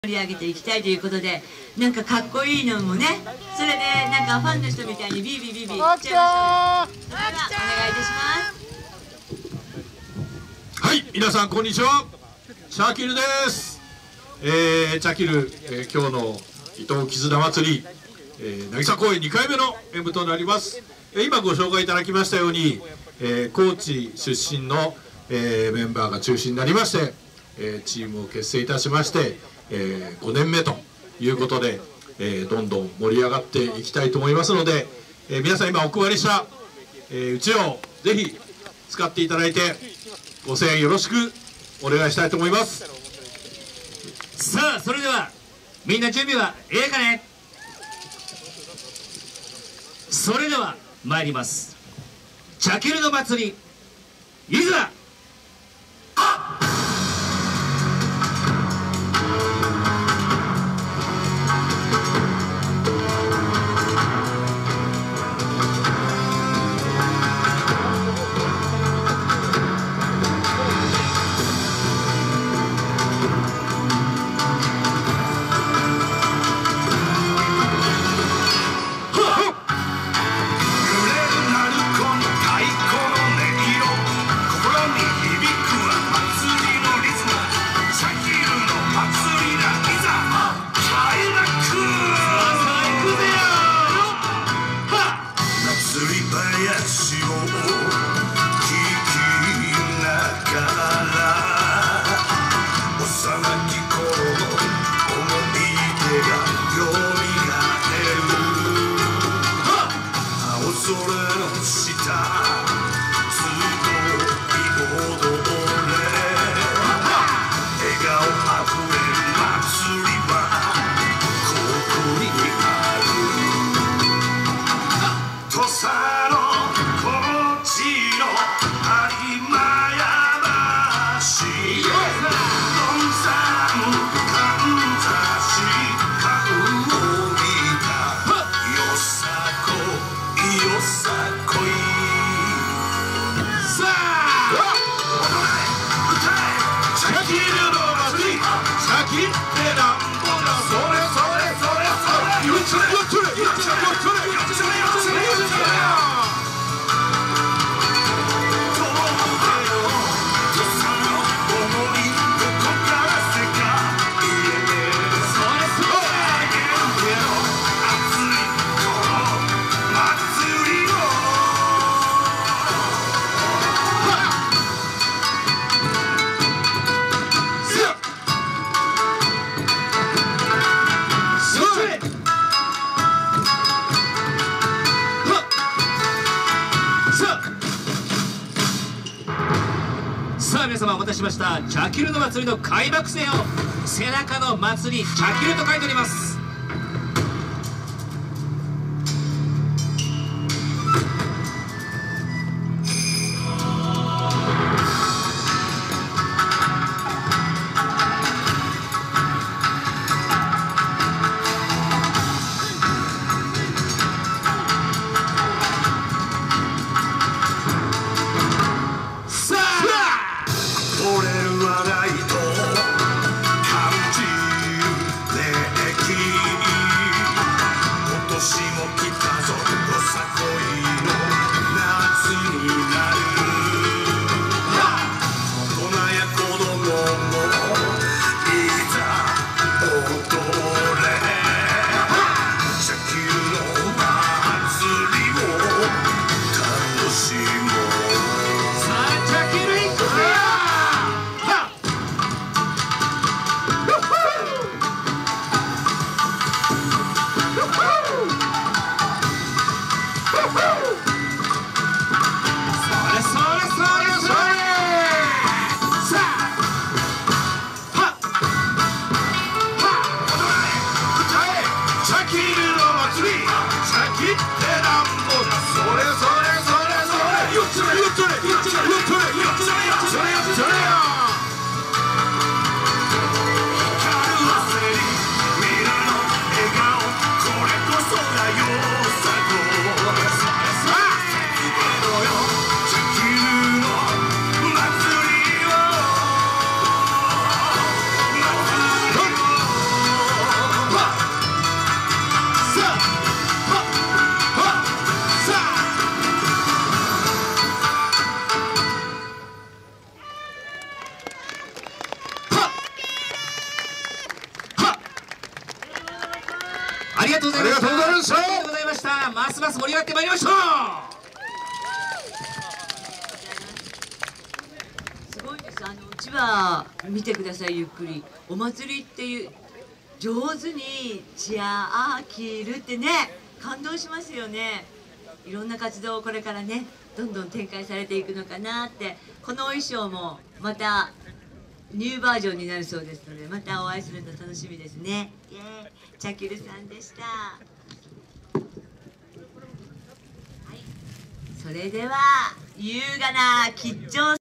ちゃきる【4K_100Mbps】／第2部／第3回伊東☆絆☆祭り
2024/03/16（令和6年3月16日）
【会場】なぎさ公園、伊東市観光会館ホール
【主催】伊東絆祭り実行委員会